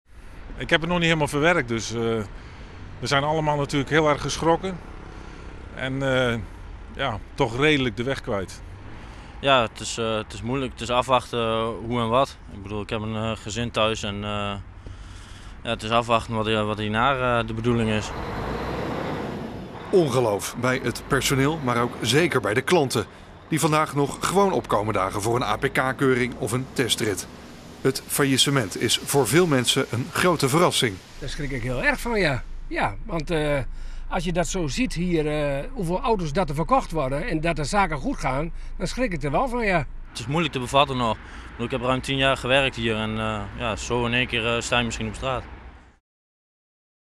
Personeel Koops Furness ontslagen in afwachting van doorstart
Het personeel van autobedrijf Koops Furness is vanmiddag ontslagen door de curatoren. Er wordt nog wel gepraat over een mogelijke doorstart.